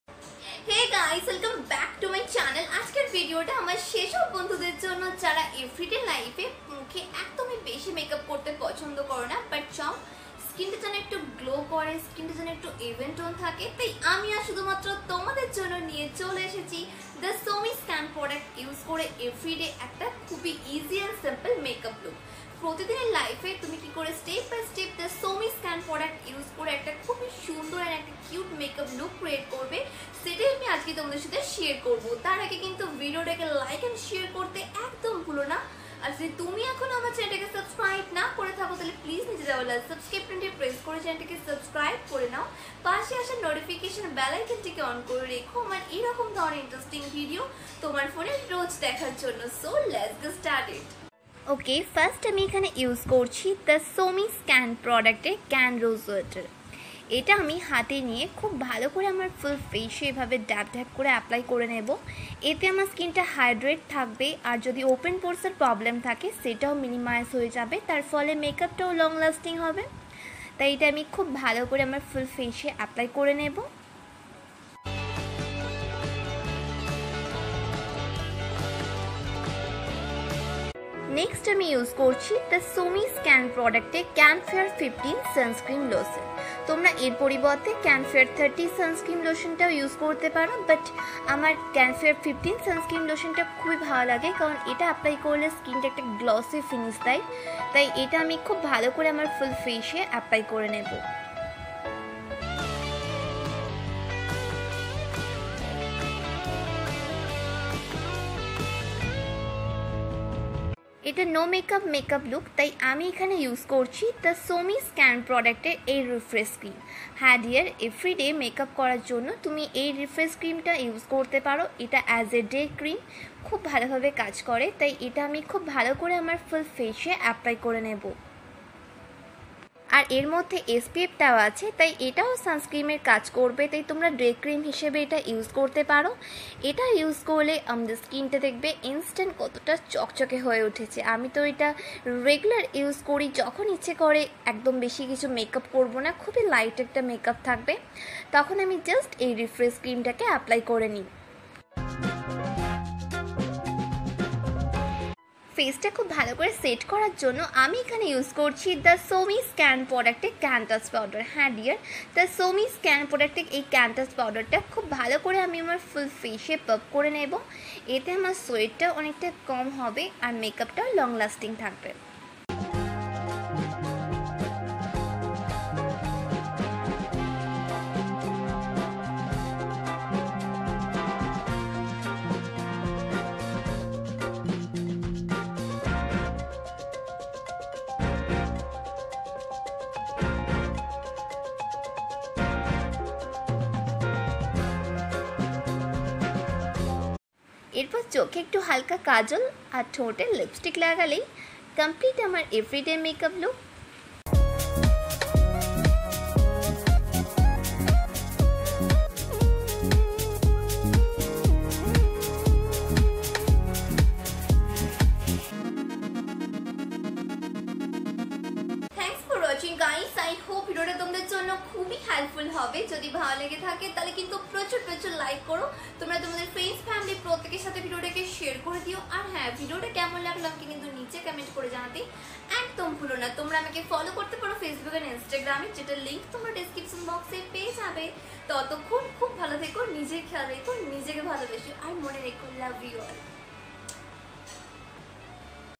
"The Soumi's Can Product" ব্যবহার করে প্রতিদিনের Easy & Simple Makeup Look // Beauty Highlighting
Hey Everyone..
Today I am sharing with u The Soumi's Can Product ব্যবহার করে প্রতিদিনের Easy & Simple Makeup Look I hope u all enjoy